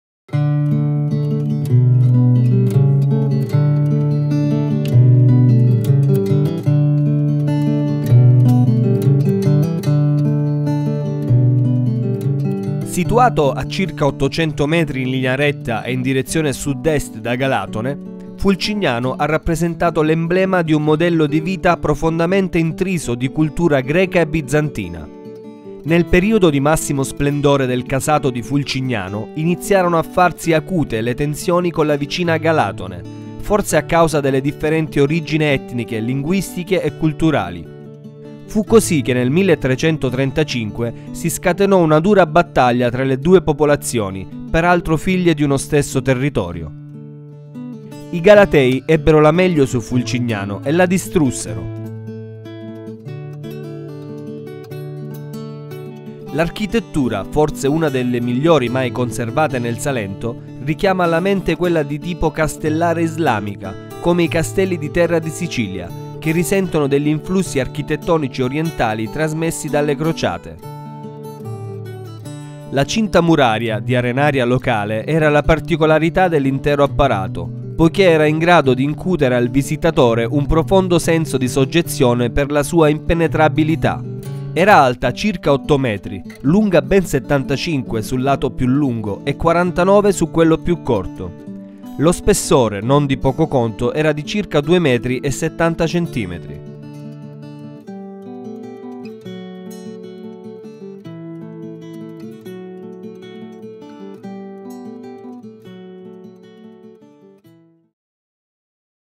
Città di Galatone - Castello di Fulcignano
Situato a circa 800 metri in linea retta e in direzione Sud-Est da Galatone, Fulcignano ha rappresentato l’emblema di un modello di vita profondamente intriso di cultura greca e bizantina. Nel periodo di massimo splendore del Casato di Fulcignano, iniziarono a farsi acute le tensioni con la vicina Galatone, forse a causa delle differenti origini etniche, linguistiche e culturali. Fu così che nel 1335 si scatenò una dura battaglia tra le due popolazioni, peraltro figlie di uno stesso territorio. I galatei ebbero la meglio su Fulcignano e la distrussero.